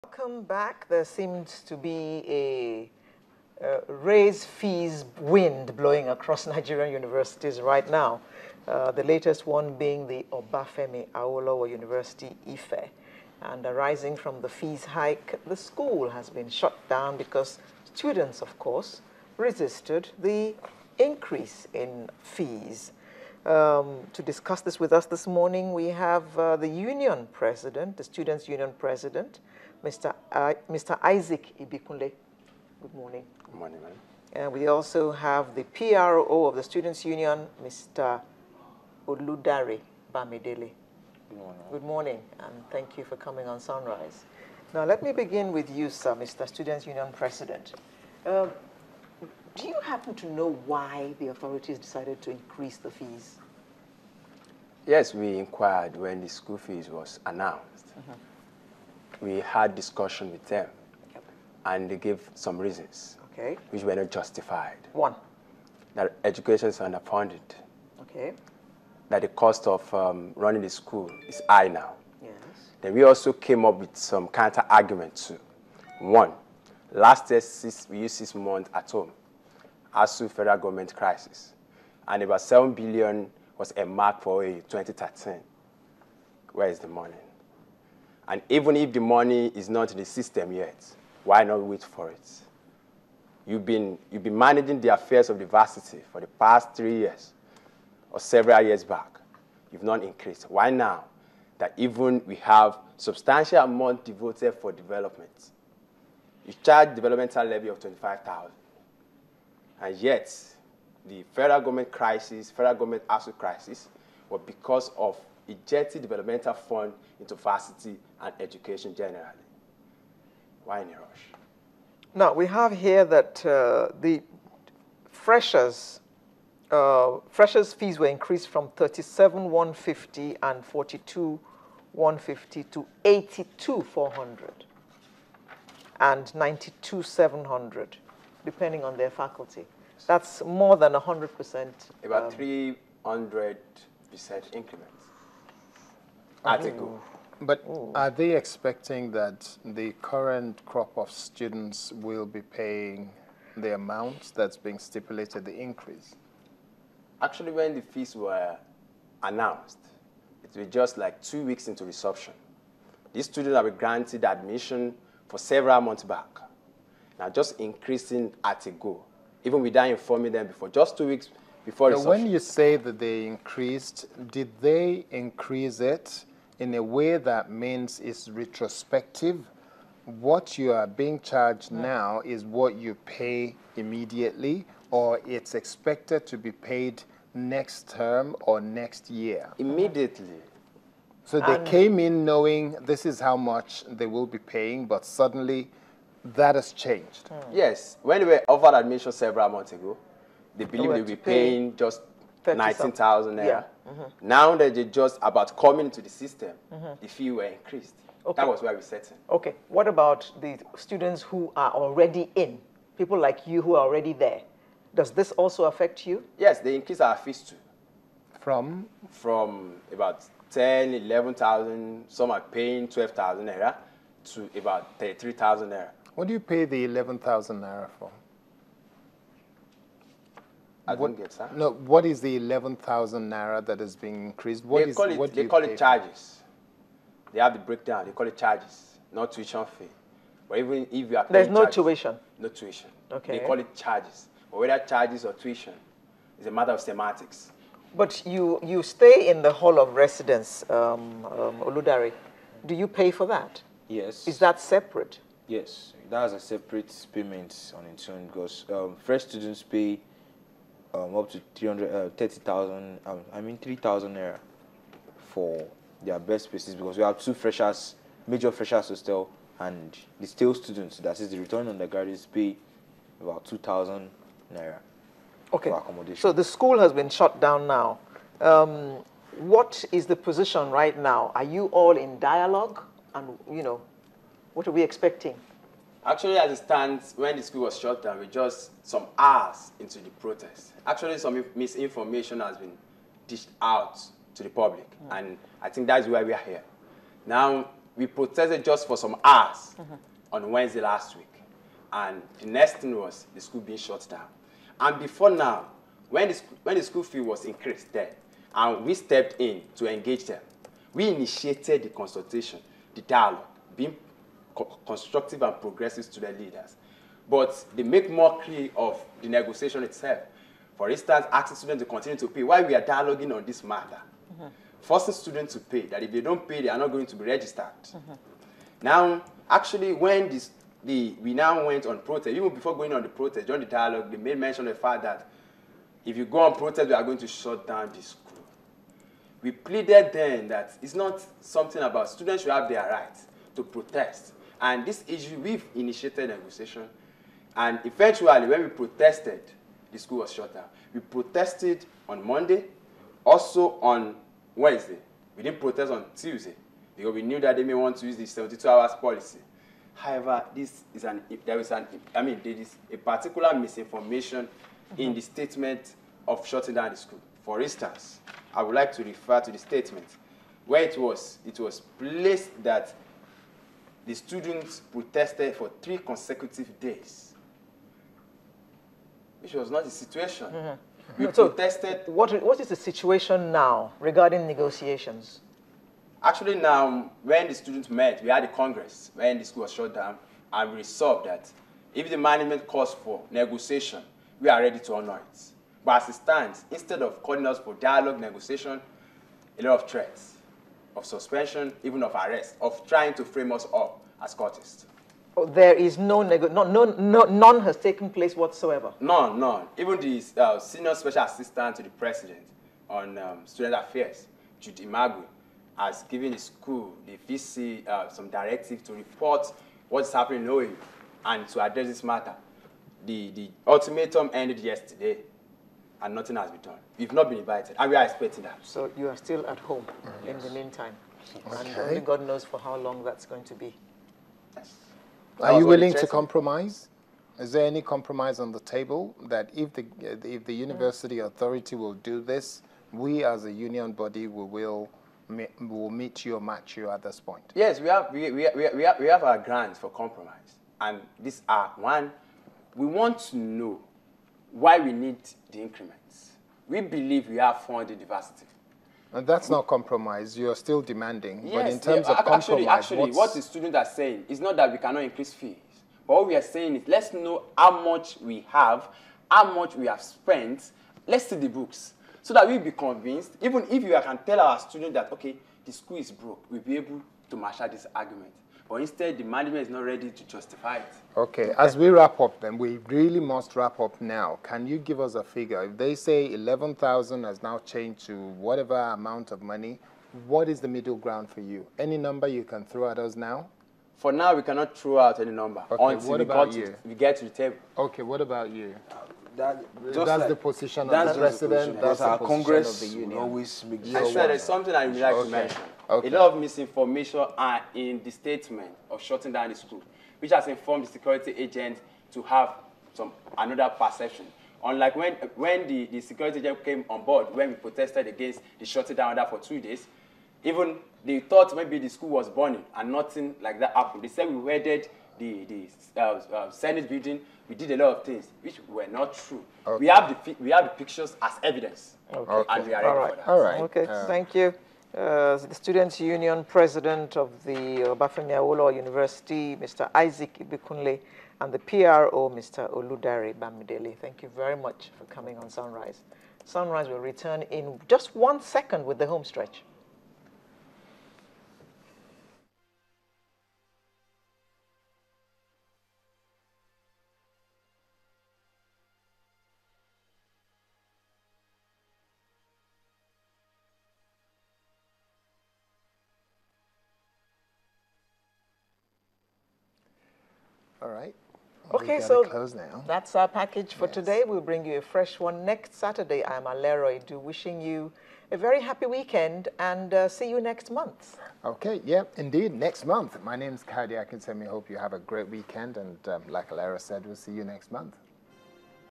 Student Leader Urges University Authority Not To Increase School Fees
The President of the Obafemi Awolowo University Student Union Government, Mr Isaac Adekunle, on Saturday urged the school authority to wait for the N7 billion allocated to them instead of increasing the school fees.
He wondered why "for the past three years they have not increased the school fees" adding that it shouldn't be at a time when "we have substantial amount of money devoted for development.